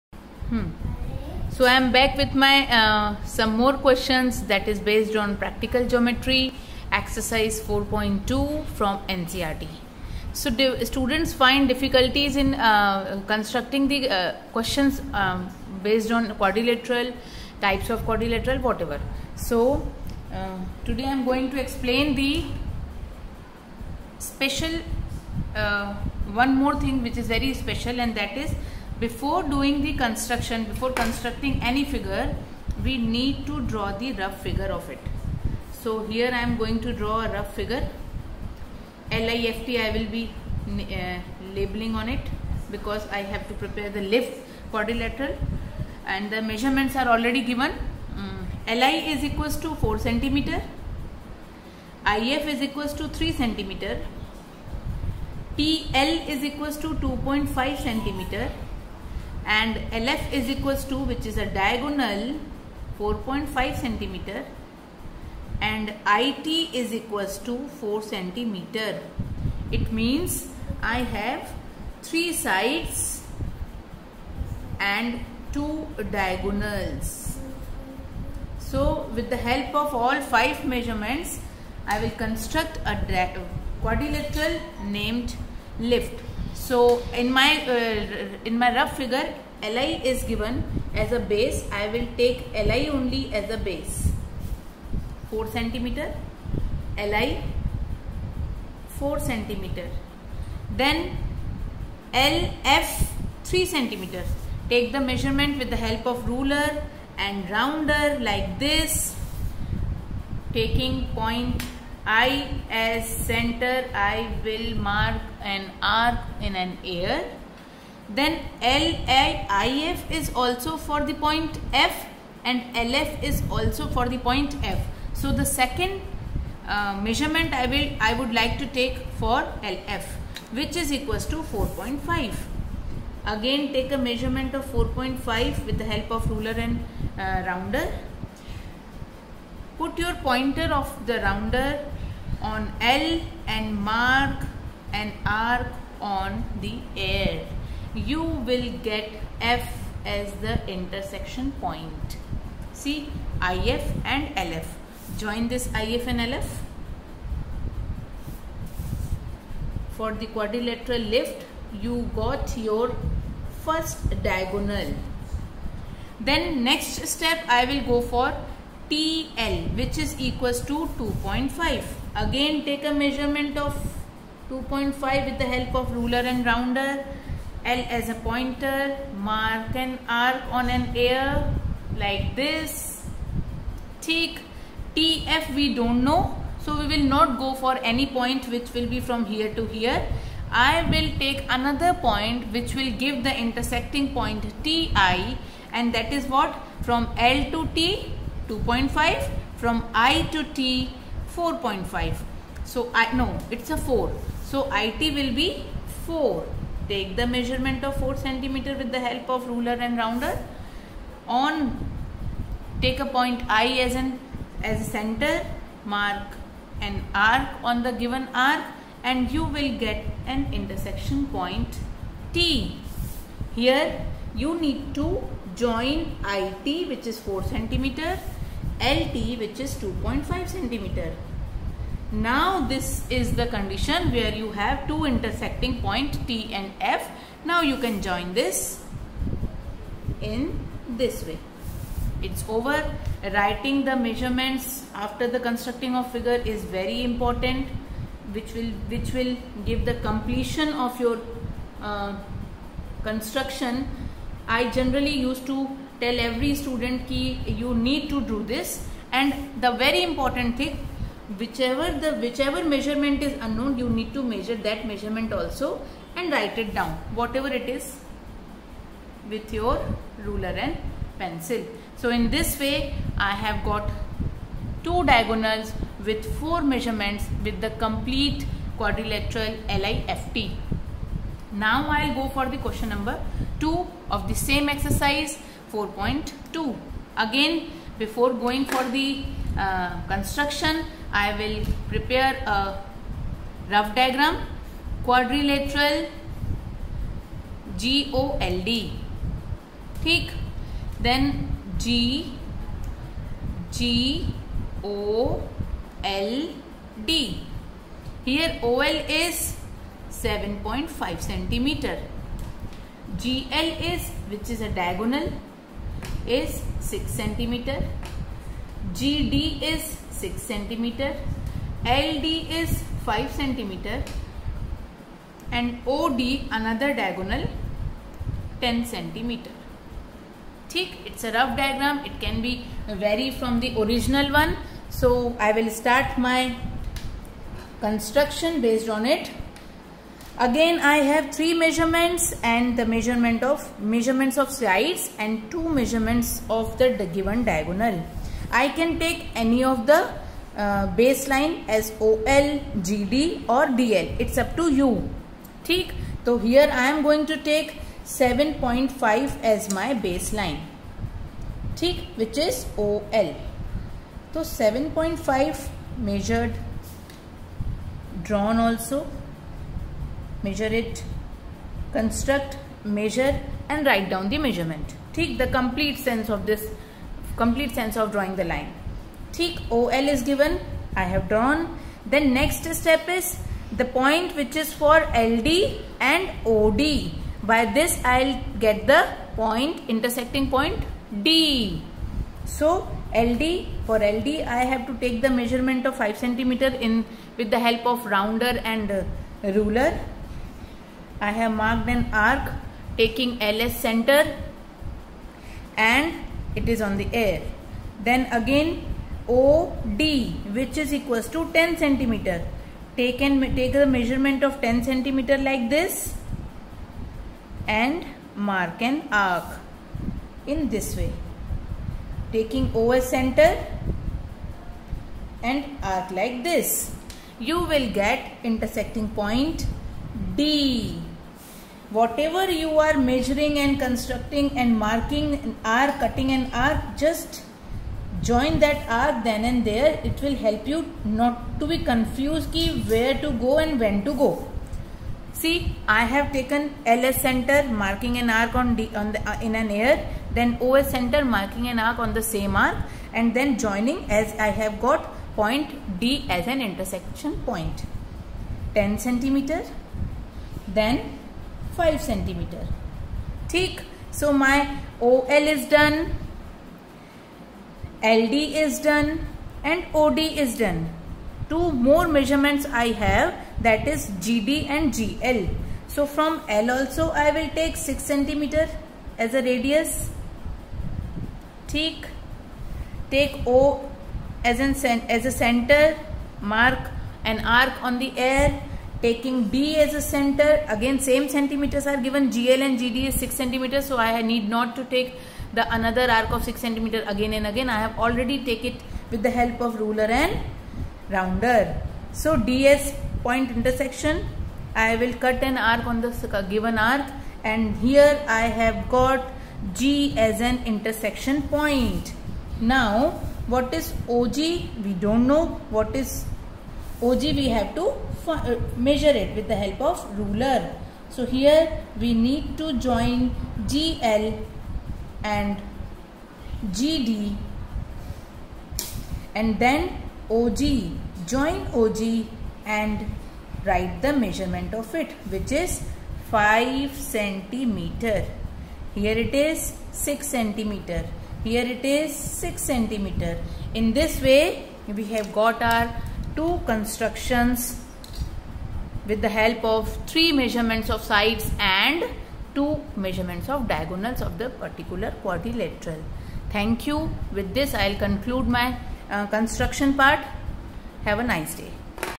CLASS VIII PRACTICAL CHAPTER 4 GEOMETRY(Construction of quadrilateral)
Ex 4.2
Construct the following quadrilaterals
(i) Quadrilateral LIFT
LI = 4 cm
IF = 3 cm
TL = 2.5 cm
LF = 4.5 cm
IT = 4 cm
(ii) Quadrilateral GOLD
OL = 7.5 cm
GL = 6 cm
GD = 6 cm
LD = 5 cm
OD = 10 cm